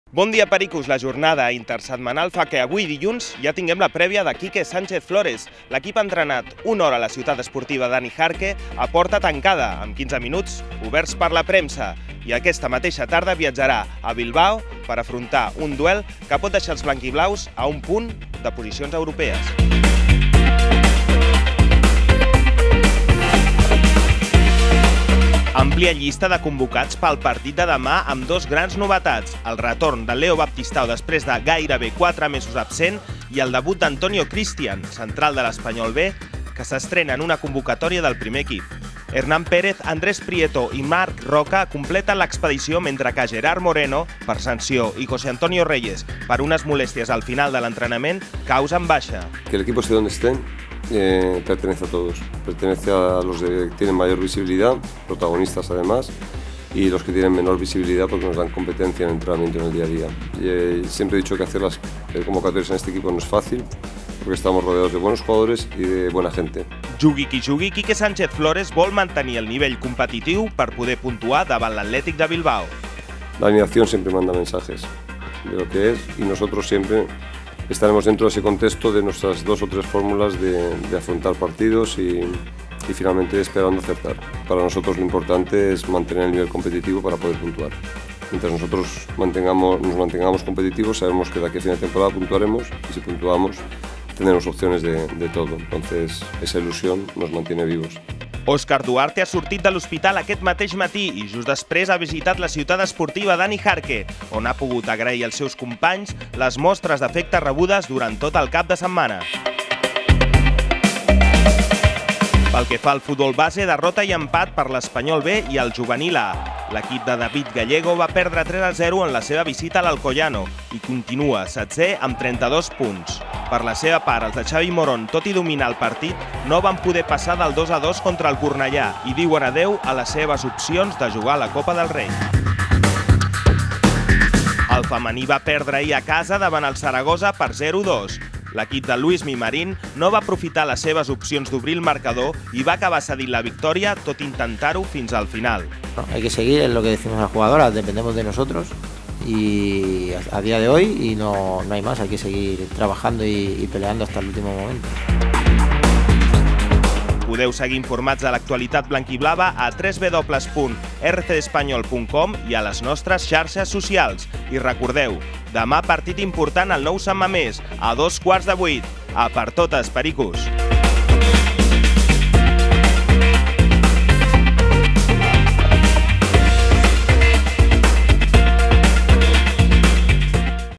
RCDE FLASH - Dilluns 3 d'abril
- Llista de convocats per a l'Athletic vs Espanyol
- Roda de premsa de Quique Sánchez Flores
- Derrotes de l'Espanyol B i el Femení i empat del Juvenil A